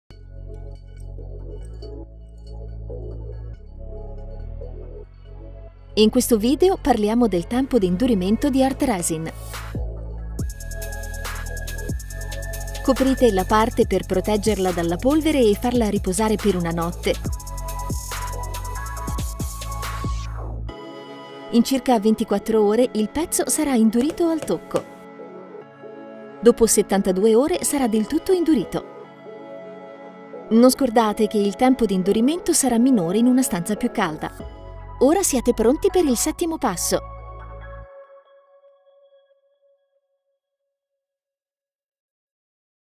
Come usare la resina epossidica ArtResin: Sesto passo - Attesa
In questo video parliamo del tempo d’indurimento di ArtResin.

Coprire la parte per proteggerla dalla polvere e farla riposare per una notte. In circa 24 ore, il pezzo sarà indurito al tocco. Dopo 72 ore sarà del tutto indurito. Non scordate che il tempo d’indurimento sarà minore in una stanza più calda.

Ora siete pronti per il settimo passo.